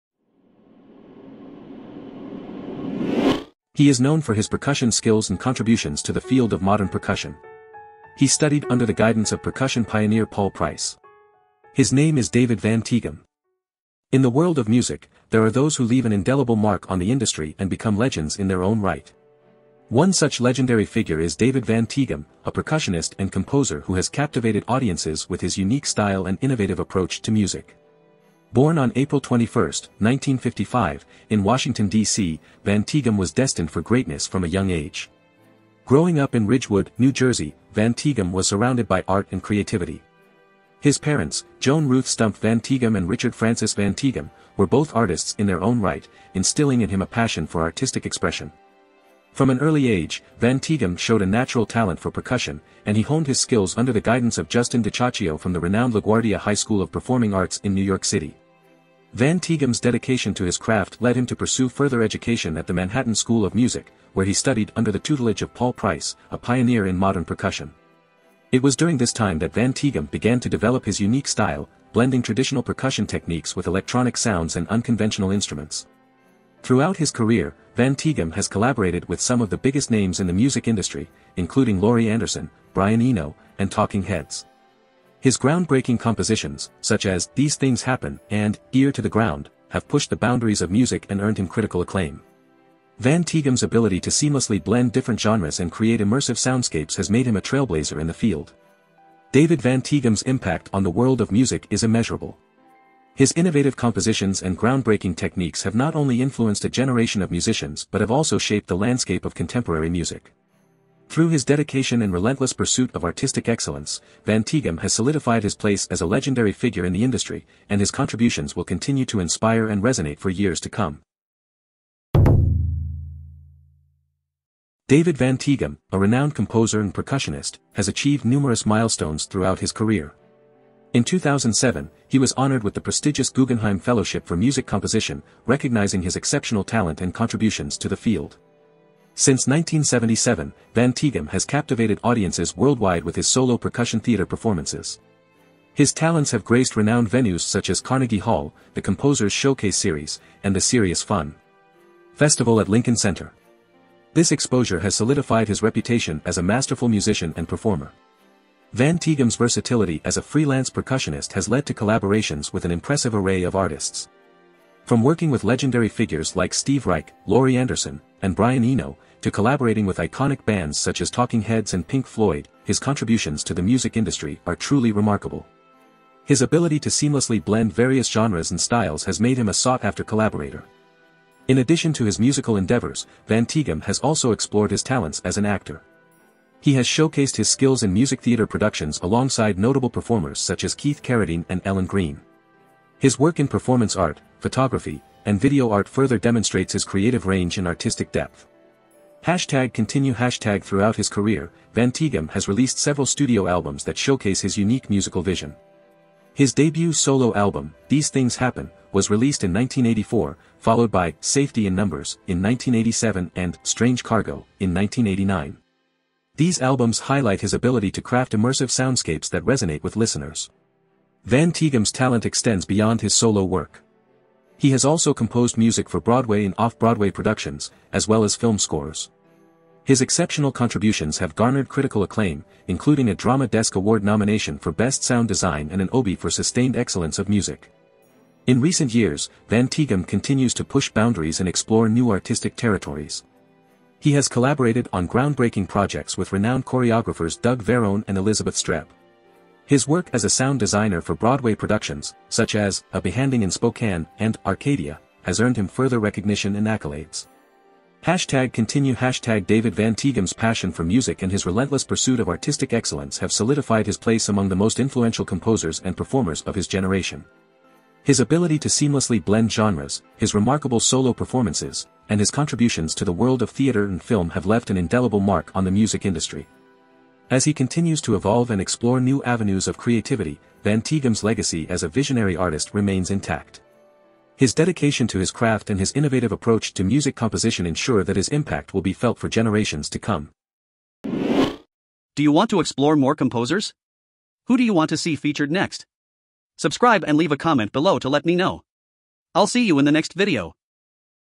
David Van Tieghem: Rhythm Maestro | Composer & Arranger Biography
David Van Tieghem was born on April 21, 1955, in Washington, D.C., and was raised in Ridgewood, New Jersey, the first son of artist and educator Joan Ruth Stumpf Van Tieghem and painter, sculptor and designer Richard Francis Van Tieghem, and brother of Richard Joseph Van Tieghem. He studied percussion with Justin DiCioccio, of LaGuardia High School of Performing Arts in New York City. He later attended Manhattan School of Music as a student of the modern percussion pioneer Paul Price. He is married to artist Cate Woodruff and they have one daughter, actress and writer Zoë Van Tieghem.